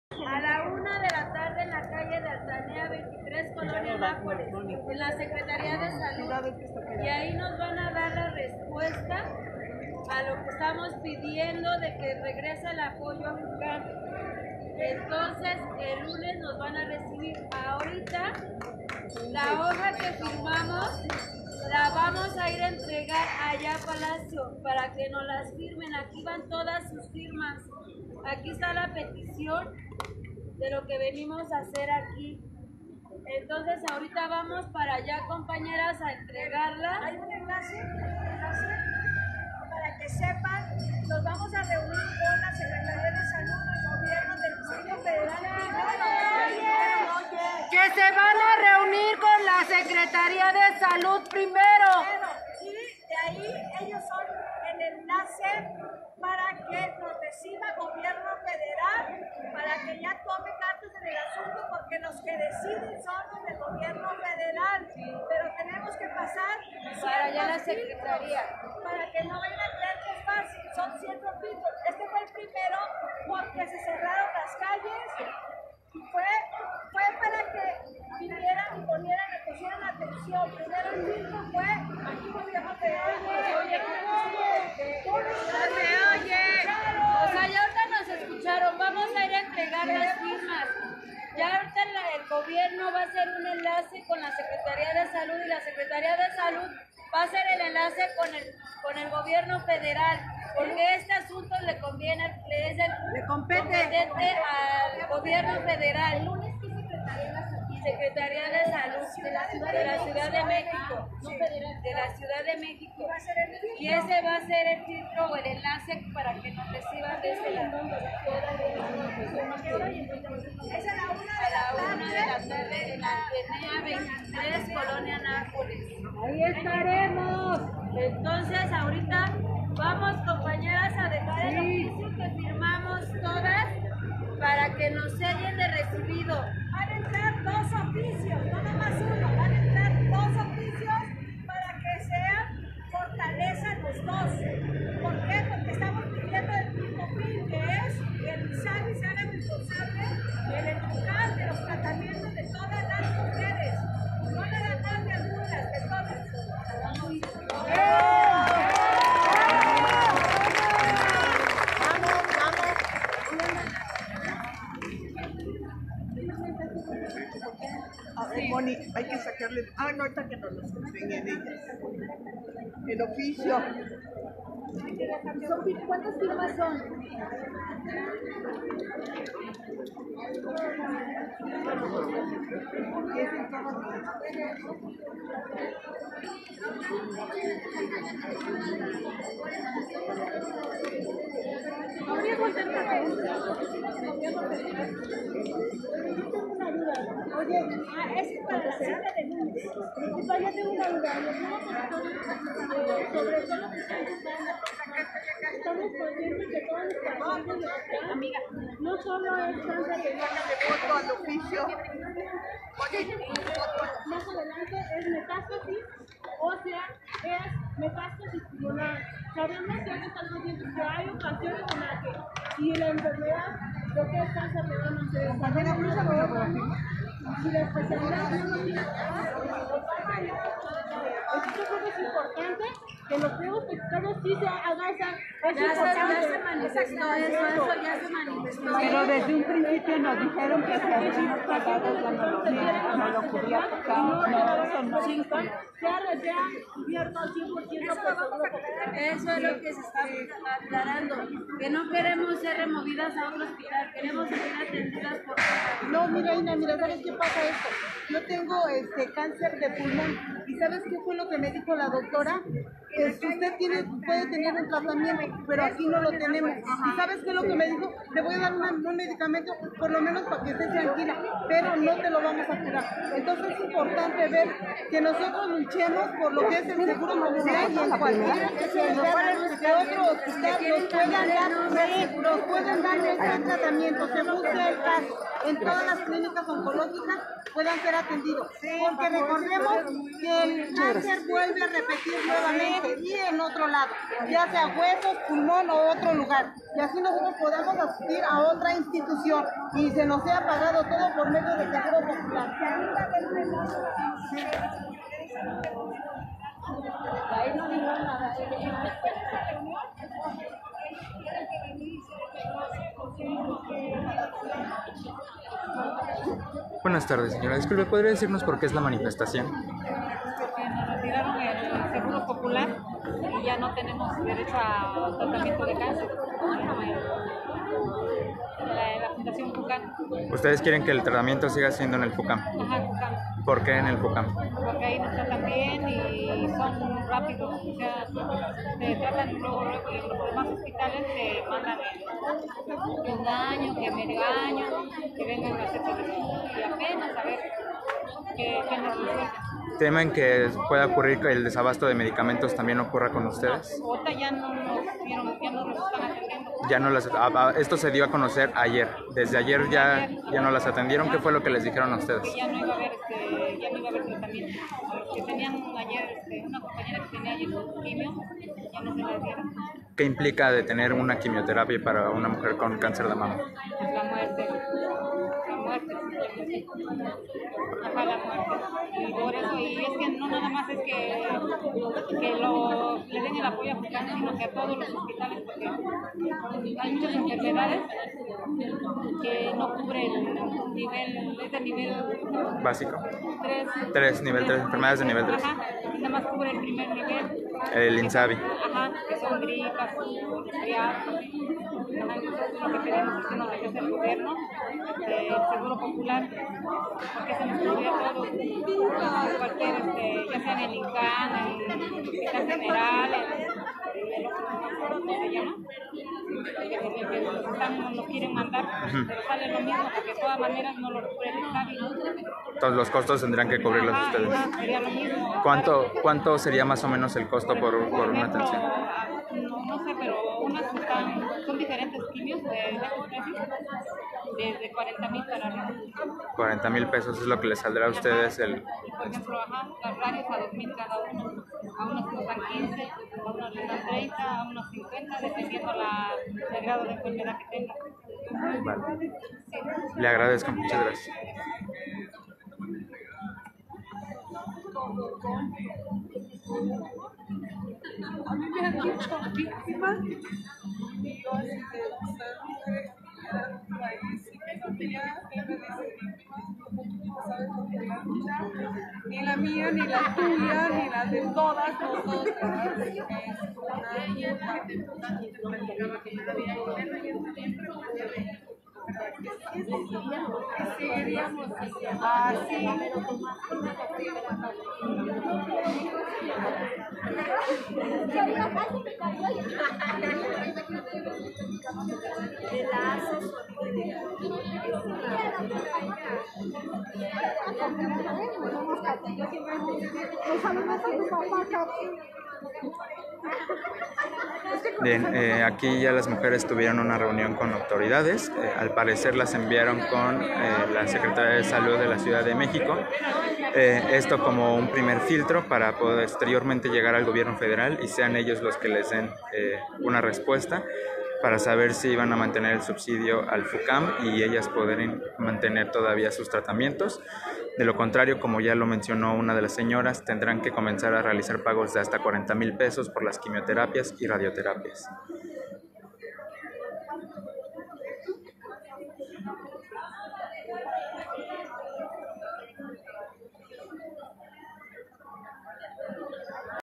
Mujeres con cáncer entregan oficio en Palacio Nacional | Contrapeso Ciudadano
El contingente que se manifestaba a las afueras de Palacio Nacional. platicaron con las autoridades y el proceso será un encuentro con la secretarioa de Salud de la CDMX.